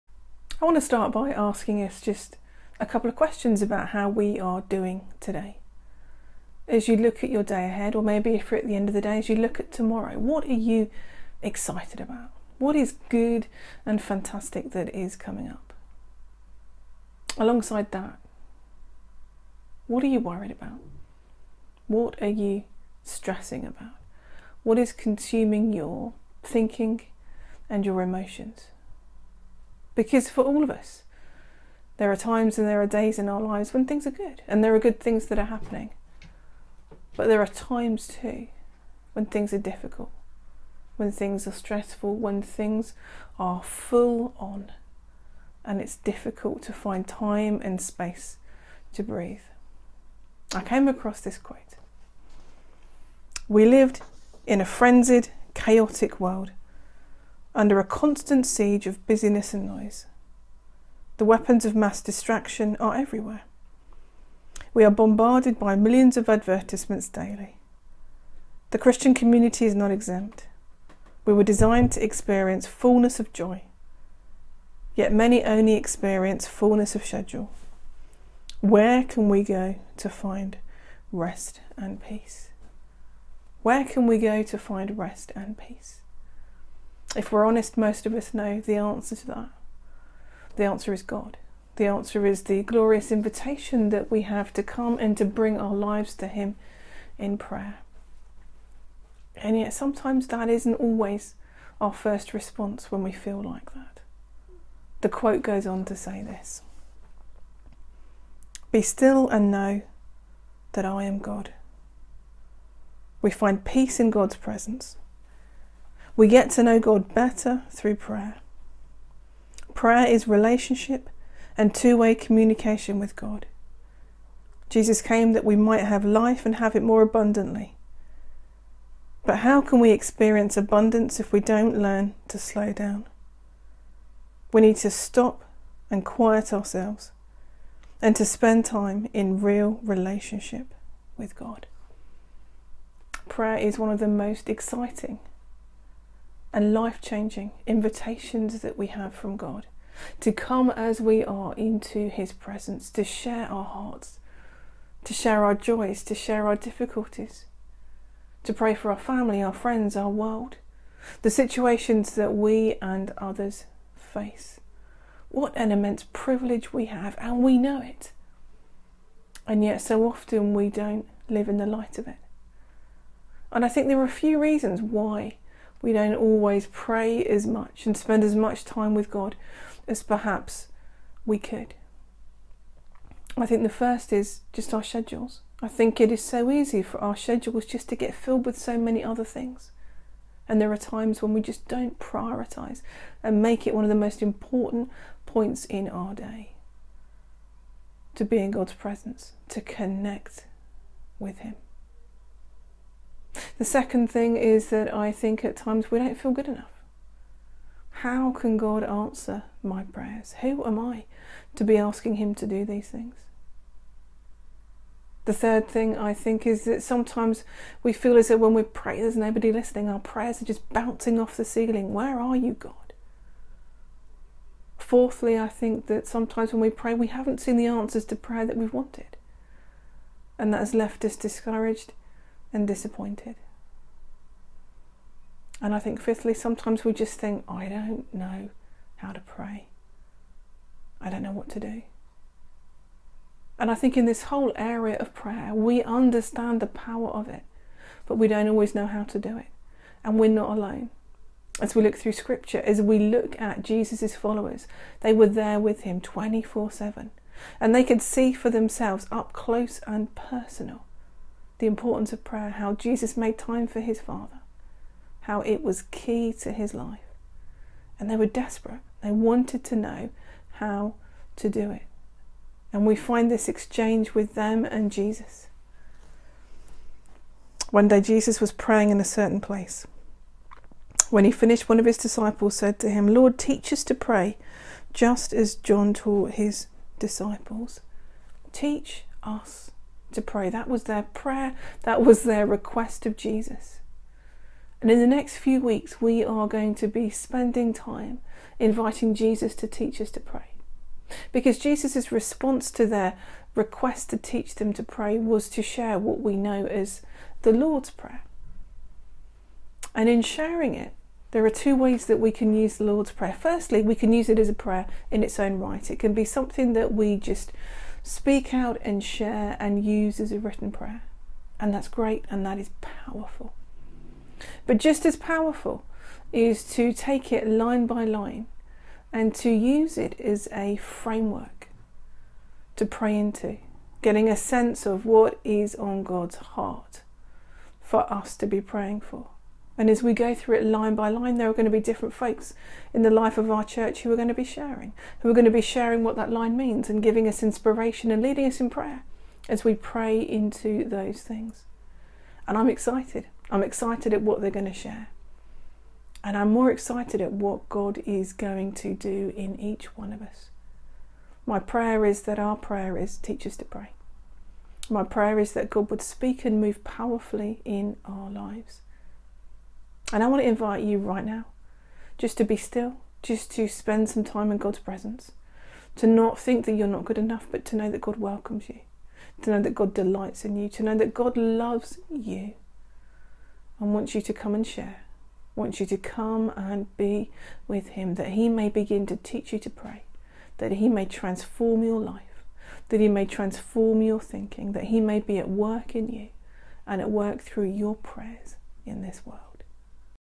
Introduction to the Lord's Prayer Series
Over the coming weeks, various folks are going to take a line each of the Lord's Prayer and help us to pray into that area whether it is for ourselves, someone else or the world. Cath leads us in an introduction to the series in this devotional.